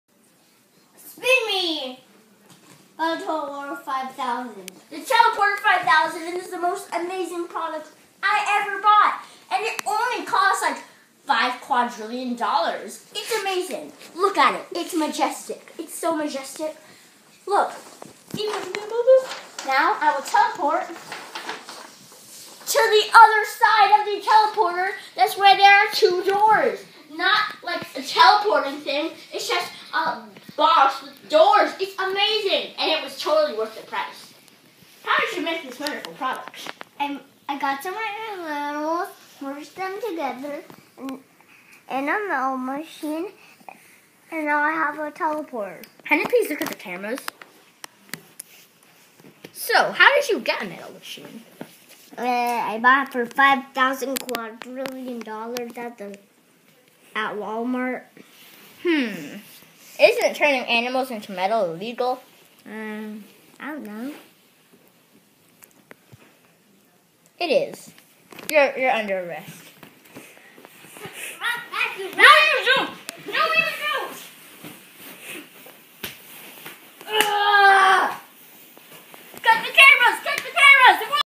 Fake teleporter creator gets arrested on camera( totally real)
Cut the cameras! cut the cameras! the world doesn't need to see this!😲